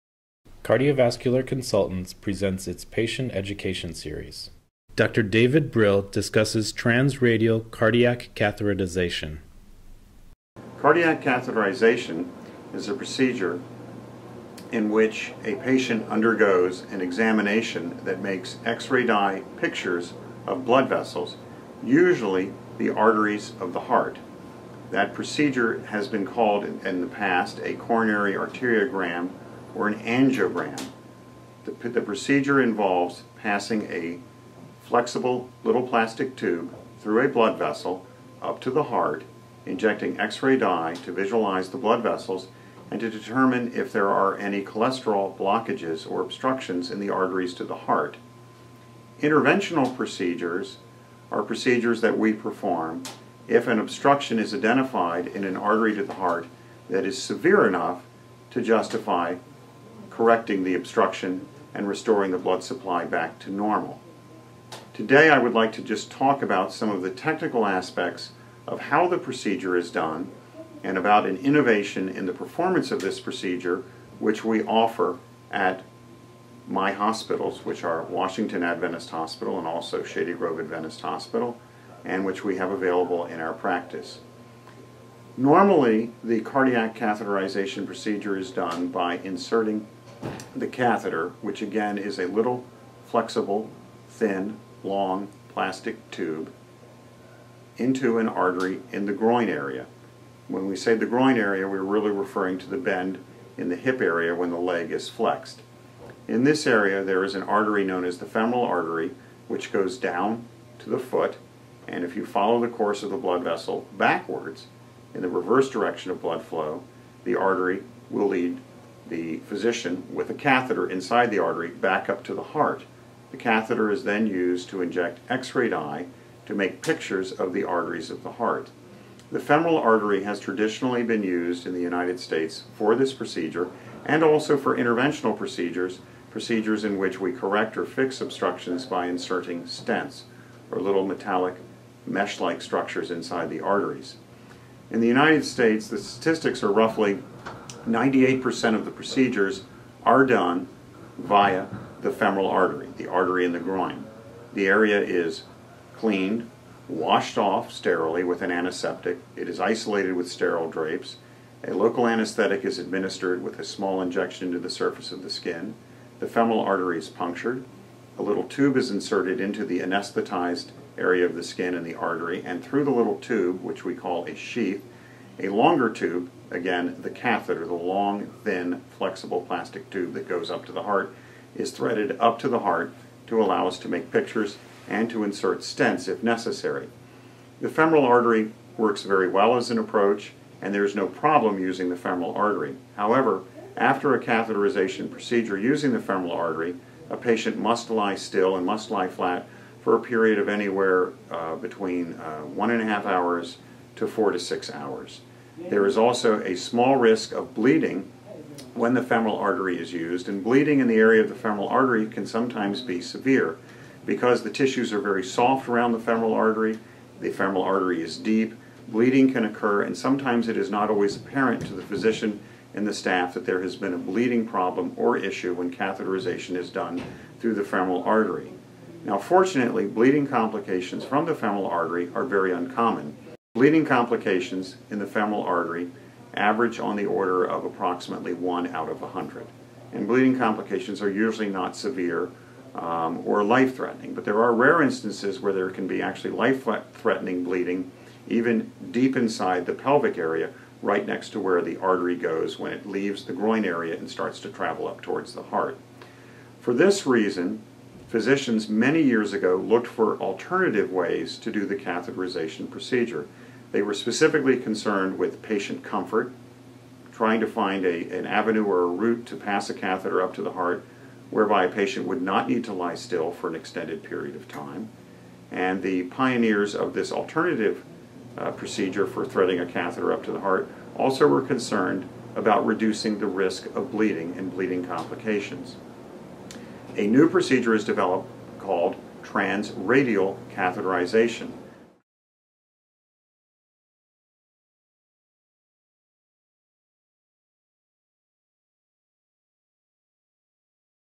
Transradial Catheterization, Part I
Dr. David Brill, Cardiovascular Consultants, P.A., Takoma Park, Maryland, discusses Transradial Catheterization.